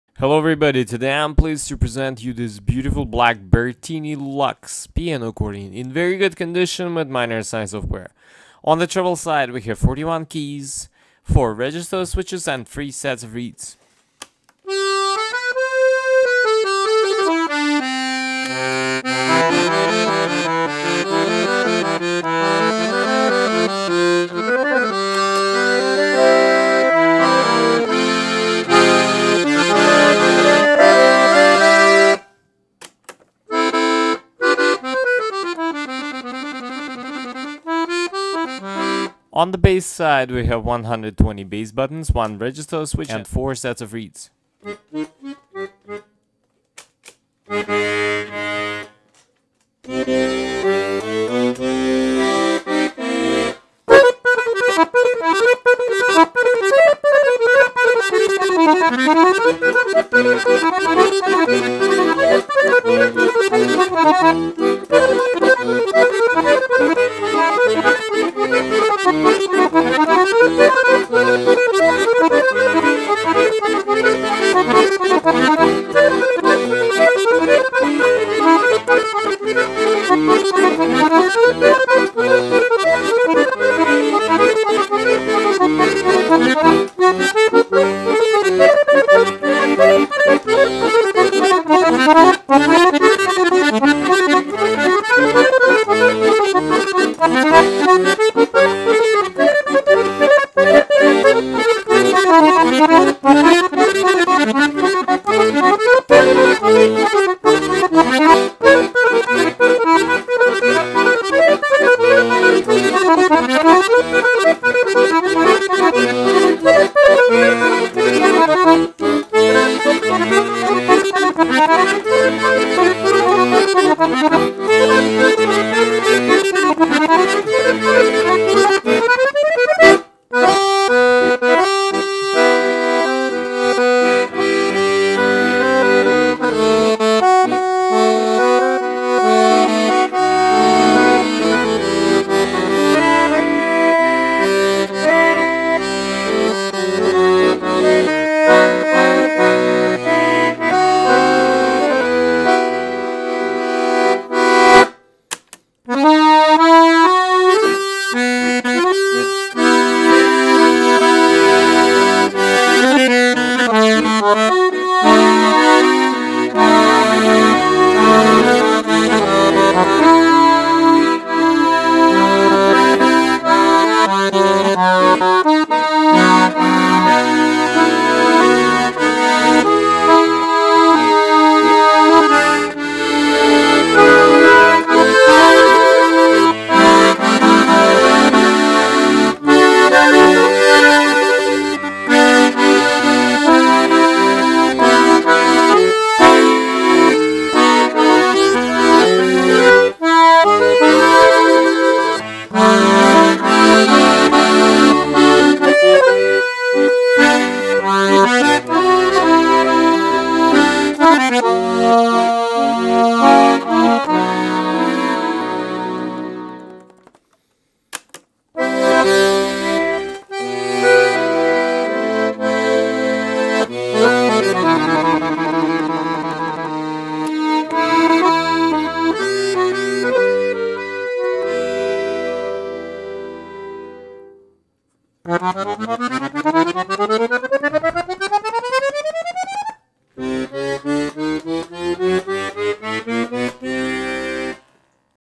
4916 - Black Bertini Lux Piano Accordion LMM 41 120 $699
Brand: Bertini
Model: Bertini Lux
Type: Piano Accordion
Reeds: 3/4 LMM
Treble: 41 Keys, 17.5"(44.5cm) Key to Key, 4 Registers
Bass: 120 Bass Buttons, 1 Registers
Weight: 14.2lbs / 6.4kg
Features: Versatile yet lightweight with demi-swing tremolo tuning
Includes: Used Straps and Case, 6 Month Store Warranty